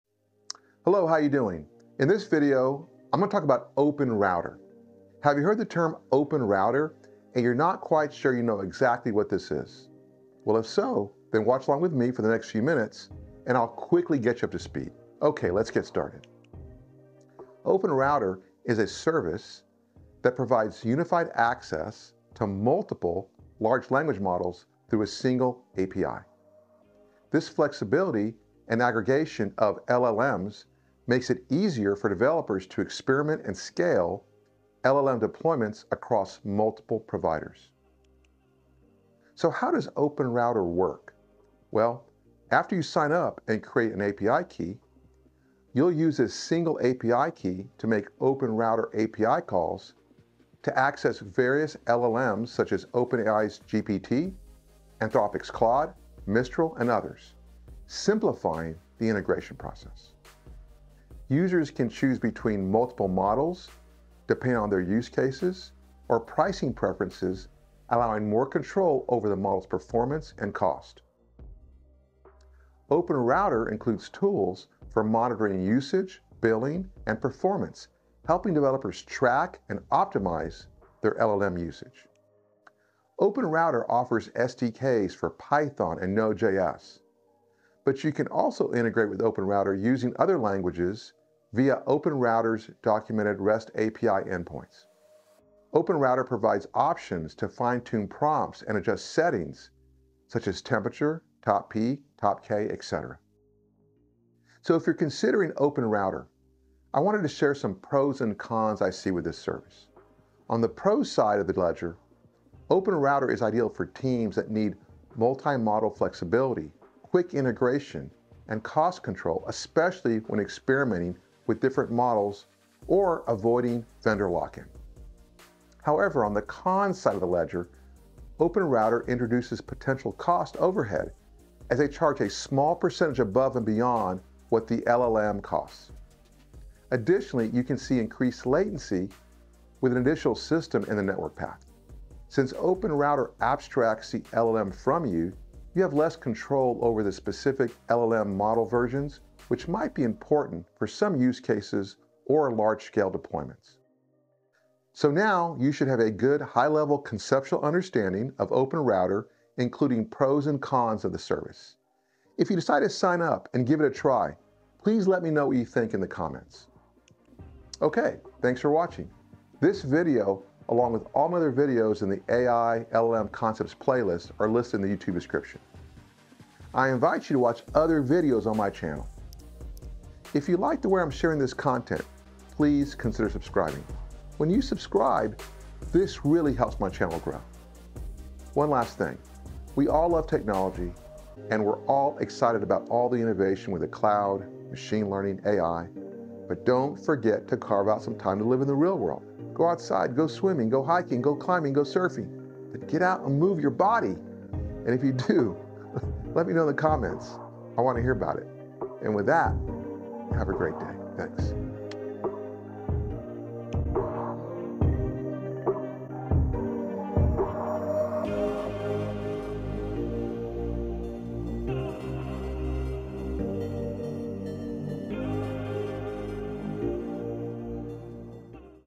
What is Open Router ?
📹 VIDEO TITLE  📹

✍️VIDEO DESCRIPTION ✍️
In this video, we’ll explore OpenRouter, a powerful tool that simplifies access to multiple large language models (LLMs) through a unified API. Whether you’re working with GPT, Claude, or Mistral, OpenRouter allows you to easily integrate and switch between different models without managing separate APIs. To get started, all you need to do is sign up, obtain an API key, and you’re ready to leverage multiple AI models from one central platform. This makes it an excellent option for teams working on various AI-driven applications and experiments.

One of the major benefits of OpenRouter is its ability to offer flexibility, cost control, and quick integration. For teams experimenting with different models, it’s a game-changer — giving you the freedom to explore, tweak, and switch between models without being locked into one provider. This is particularly useful for managing costs while optimizing performance, as OpenRouter allows you to experiment with a variety of LLMs under a single API. It’s an ideal solution for organizations that need the versatility of multiple models while keeping integration simple and streamlined.

However, OpenRouter does come with a few considerations. First, there’s the potential for cost overhead, as OpenRouter adds a small percentage charge on top of the base model costs. This may increase your overall budget if you’re scaling up. Additionally, introducing another system in your network path can result in increased latency, which might impact performance in time-sensitive applications. Finally, by abstracting the models, OpenRouter gives you less control over specific versions of LLMs, which could be a limitation for large-scale deployments or use cases requiring exact model management.

🧑‍💻GITHUB URL 🧑‍💻
No code samples for this video